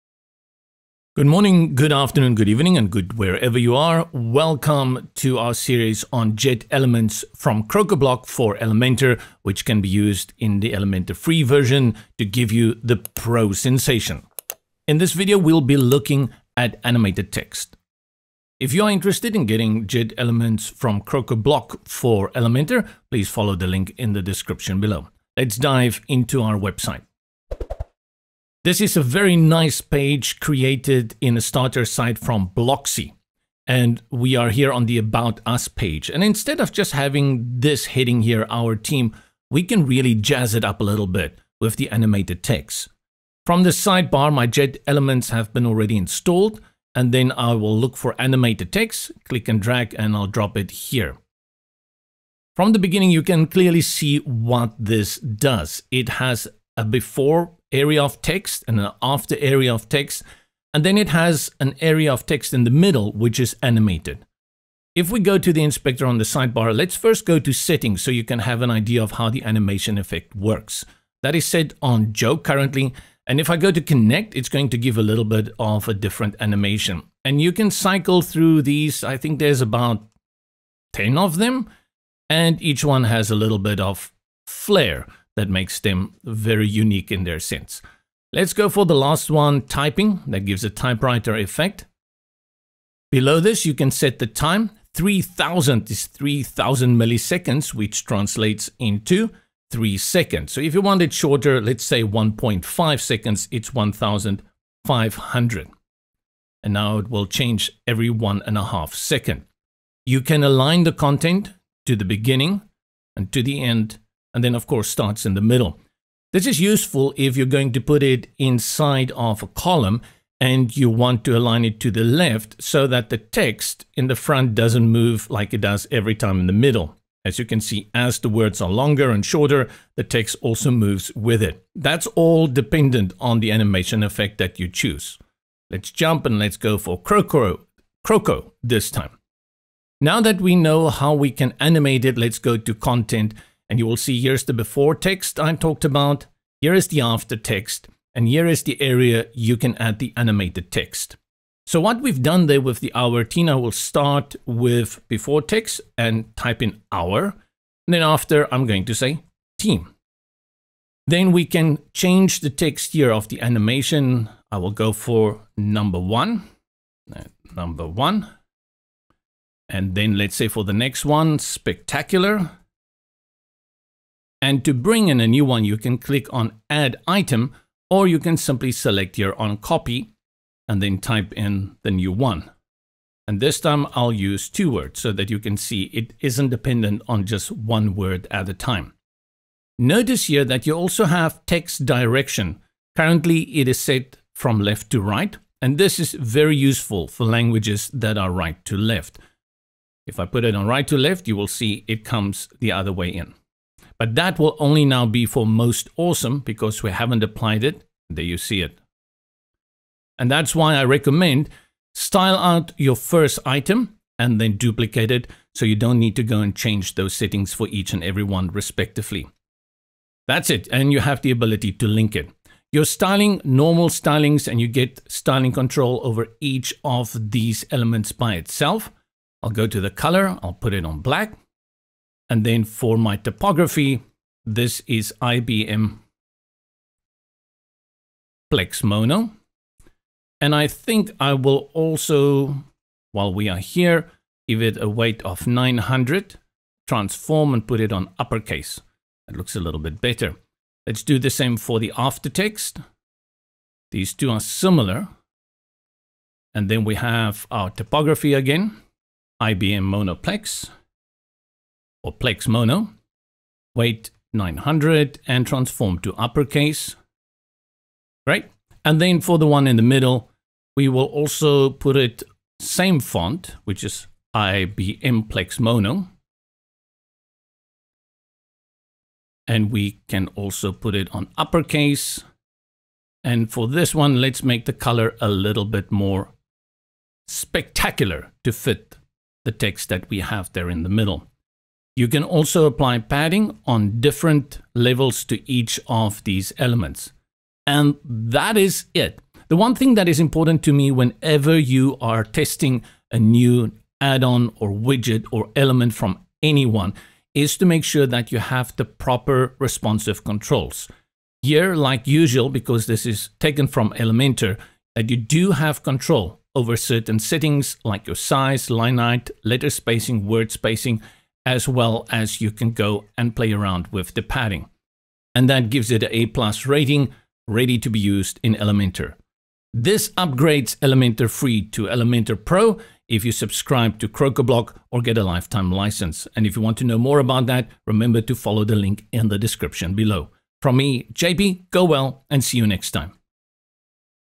Animated Text for Eye Catching Headings | Elementor, Crocoblock -- JetElements 2022, Best of
Add flare to headings and have them stand out against the competition with JetElements from Crocoblock for both Elementor FREE and PRO.